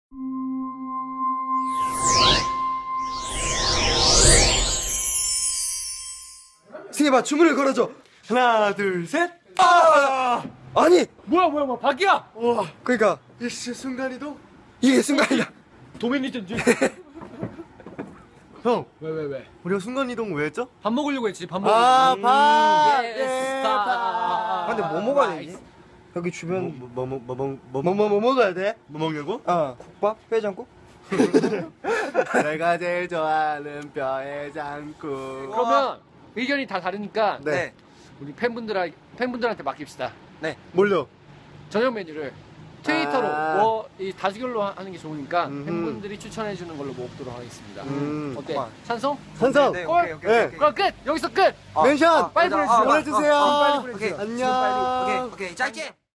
A-JAX 깨알TV - 저녁 메뉴를 정해주세요!
승엽, 승진, 성민의 저녁메뉴를 정해주세요! 영상을 보시고 트위터에 메뉴를 지금 바로 남겨주세요~ 그 중 하나를 선택해서 저녁식사를 할 예정이니 많은 댓글 부탁드려요~ ;)